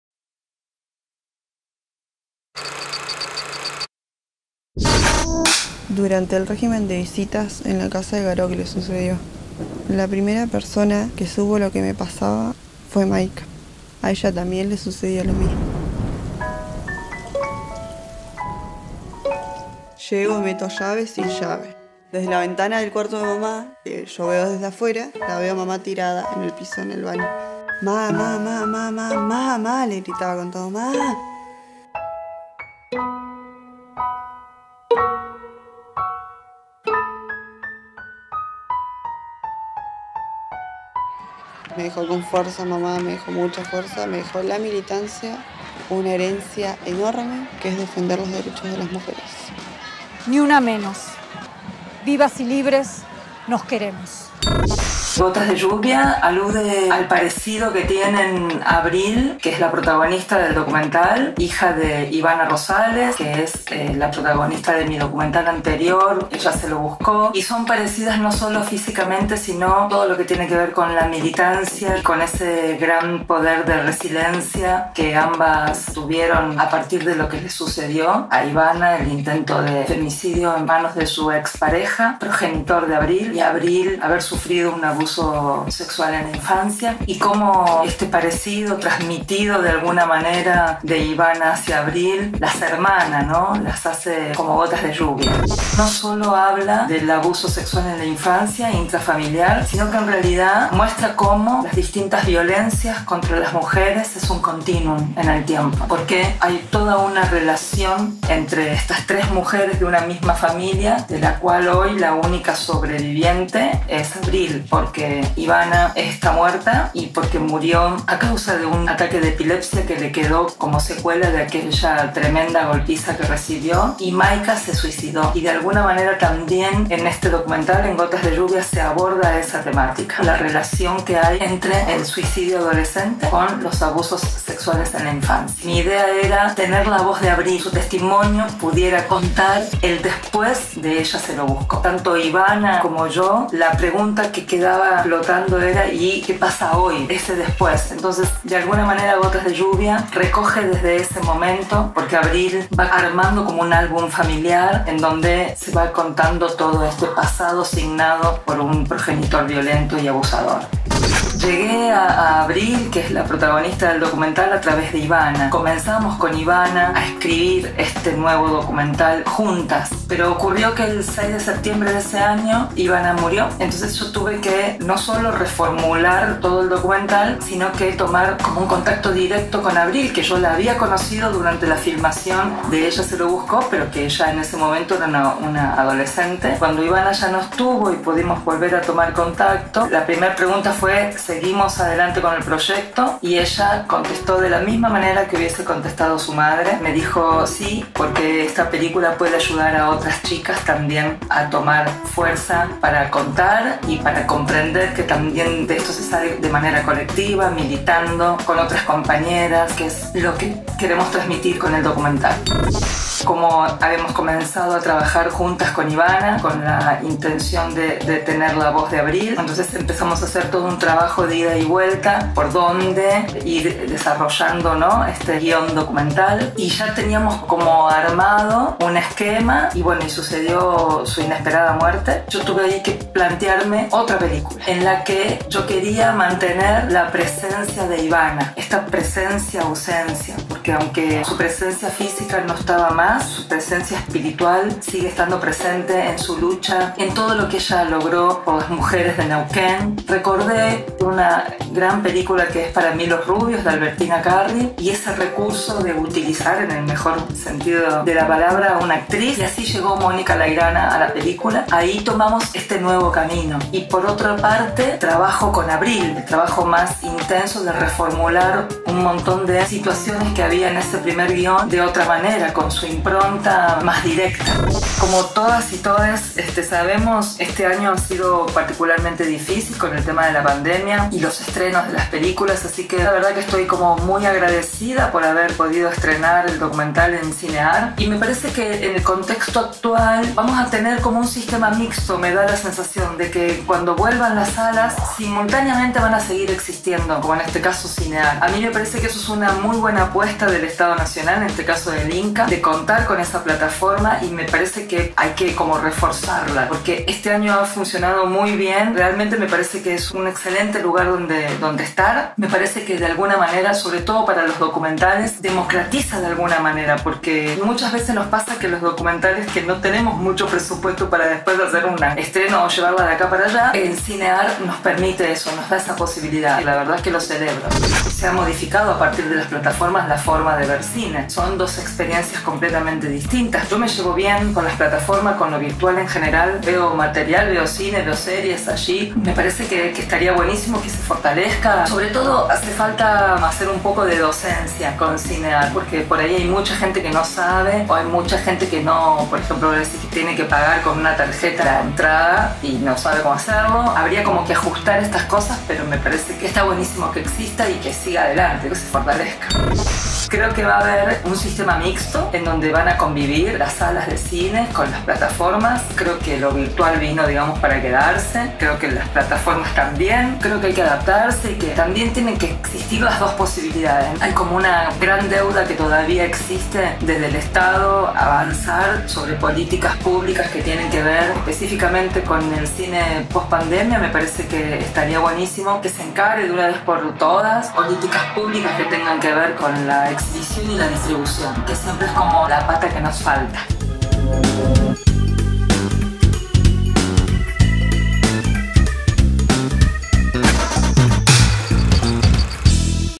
Entrevista a Susana Nieri
La directora Susana Nieri presenta Gotas de Lluvia, documental que ahonda en problemáticas de género a partir de los abusos y situaciones de violencia sufridos por mujeres.